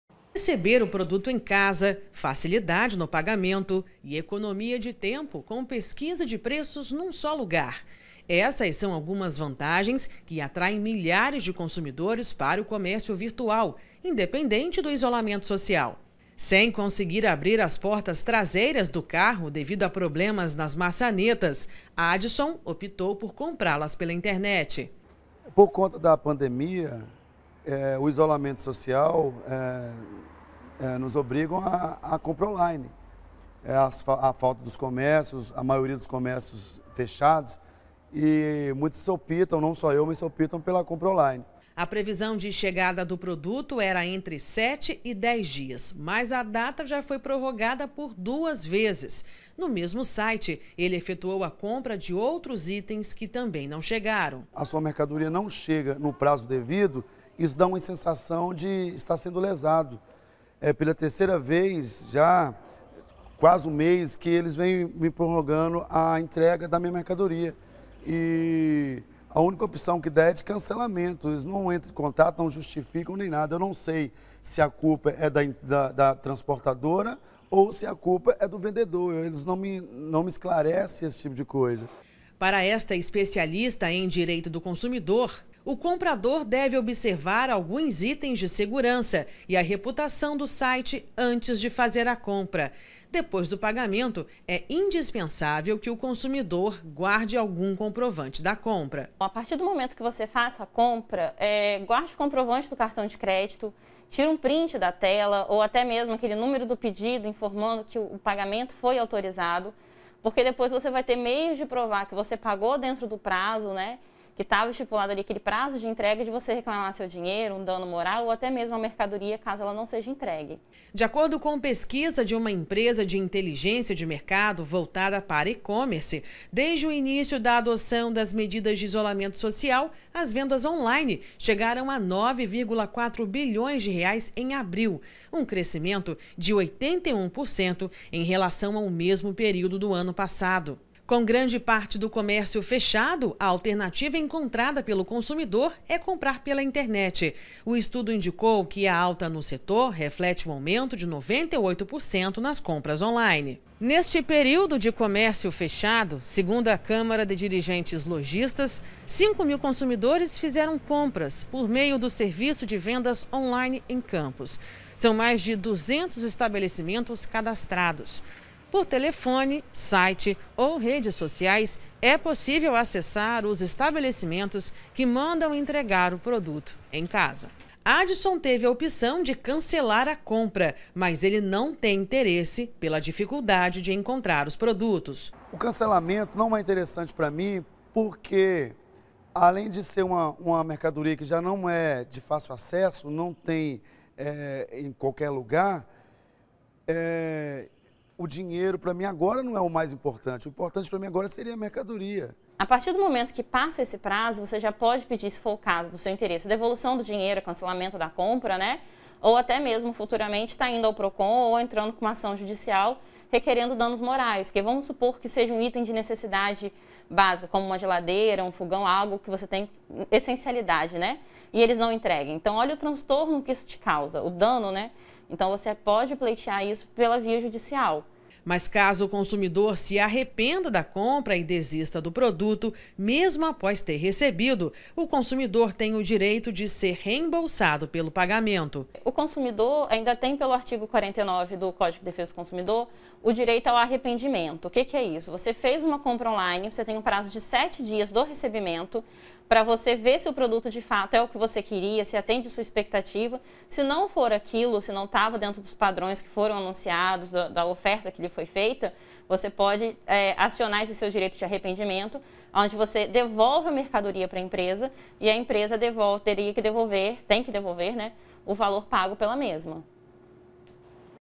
Vendas on-line cresceram mais de 80% na pandemia
Com o isolamento social, muitas pessoas estão fazendo compras pela internet para ter a comodidade de receber produtos em casa. Pesquisas apontam que as vendas on-line cresceram mais de 80% neste período de pandemia do novo coronavírus.